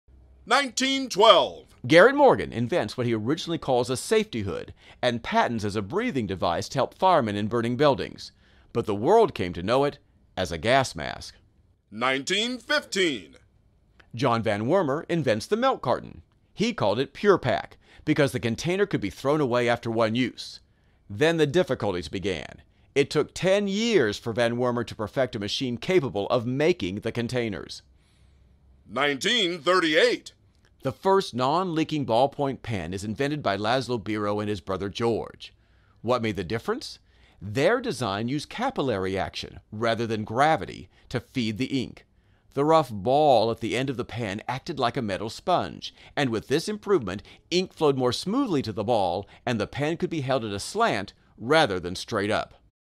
When Did That Happen? Interesting Inventions, Part 4
Learn about the invention of the gas mask, milk carton, and ballpoint pen.    @fpclassroom